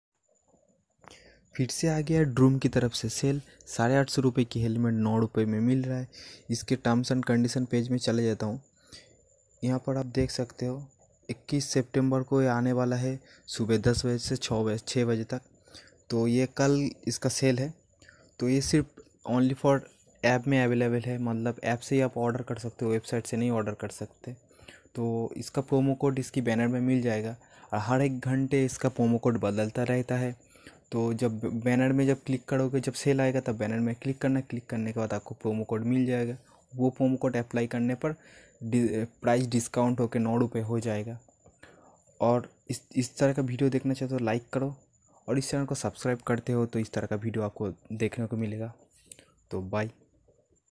Droom flash sale Motor Bike Helmet just RS 9 only (On 21 SEPTEMBER @10 AM)
Droom flash sale On 21 SEPTEMBER @10 AM

Droom Flash Sale On Gloves:-  Droom Online Motorplace Has Come Up Again With Flash Sale On helmet . Under Which You Will Get Motor Bike helmet Worth Rs 850 At Just Rs 9 Only On On 21 September 2021 @ 10:00 AM To 6:00 PM. Follow Below Steps To Claim Your Rs. 21 Gloves From Droom helmet Flash Sale.

This offer is organized by Droom Technology Private Limited (“Company”) for the end

Droom Helmet Sale Live.

Promcode Active Hone pr auto apply ho jayega.. Apko kuch lgana nahi pdega.

Generally niche jo de rha hu vhi lgta hai.

NEWHELMETRS9

har hour last me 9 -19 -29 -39 aise change kr dena.

NEWHELMETRS19

NEWHELMETRS29